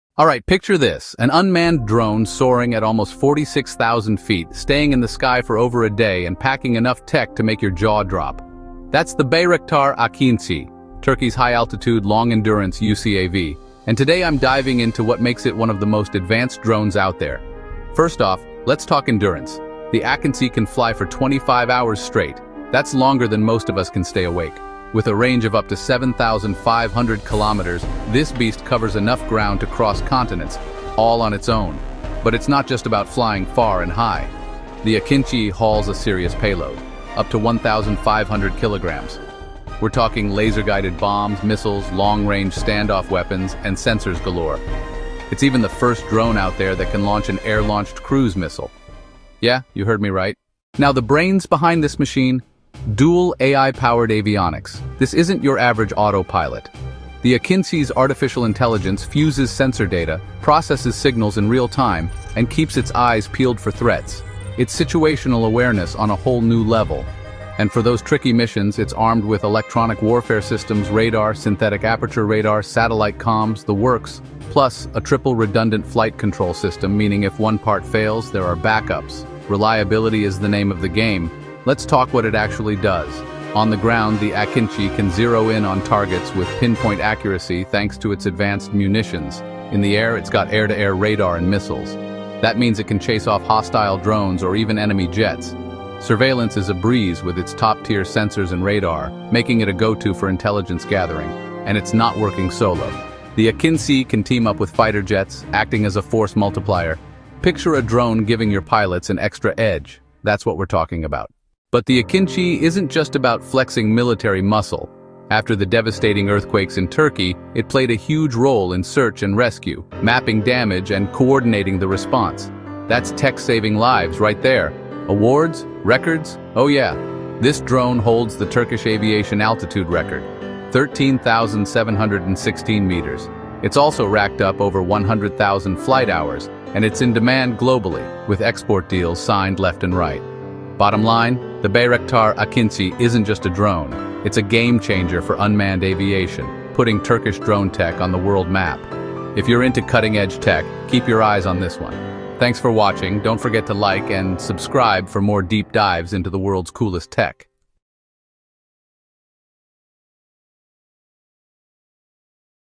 Bayraktar AKINCI Revolutionizes Combat Drone Warfare!
Discover the revolutionary Bayraktar AKINCI, Turkey’s cutting-edge high-altitude long-endurance (HALE) unmanned combat aerial vehicle (UCAV). With an impressive 7,500 km range and 25-hour flight time, AKINCI redefines drone capabilities. Equipped with a 1,500 kg payload, advanced AI-powered avionics, and versatile weaponry—including the world’s first air-launched cruise missile (ALCM)—this drone excels in air-to-ground, air-to-air, and reconnaissance missions. Its triple redundant flight controls, electronic warfare systems, and radar technologies ensure unmatched reliability and combat readiness. Notably, the AKINCI holds Turkey’s national altitude record and has played a vital role in disaster response and global exports. Witness the future of aerial warfare with this tech marvel!

In this groundbreaking video, we explore the game-changing capabilities of the Bayraktar AKINCI, the latest addition to the Turkish defense industry's arsenal of combat drones. With its advanced features and cutting-edge technology, this drone is set to revolutionize the face of modern warfare. From its impressive range and endurance to its advanced sensors and precision-strike capabilities, we dive into the details of what makes the Bayraktar AKINCI a force to be reckoned with. Join us as we examine the implications of this drone on the battlefield and what it means for the future of combat operations.

OUTLINE:

00:00:00 The Ultimate Combat Drone Unveiled
00:00:50 Advanced Technology and Combat Operations
00:01:51 Humanitarian Impact and Global Recognition